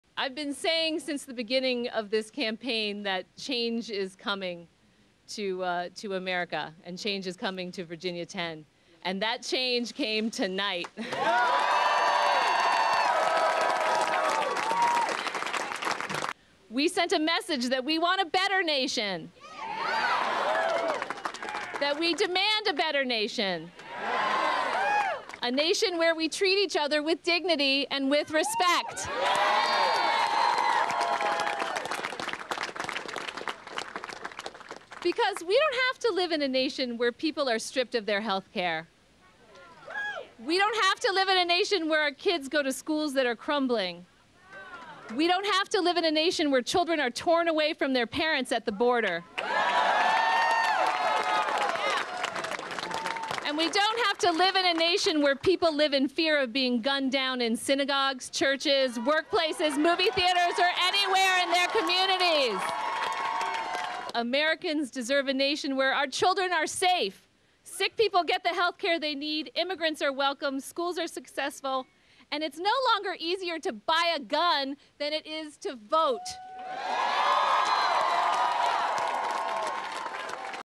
Democrat Jennifer Wexton Flips GOP House Seat in Virginia
Nov.06 -- Democrat Jennifer Wexton speaks after defeating incumbent Republican Representative Barbara Comstock in a suburban Virginia district.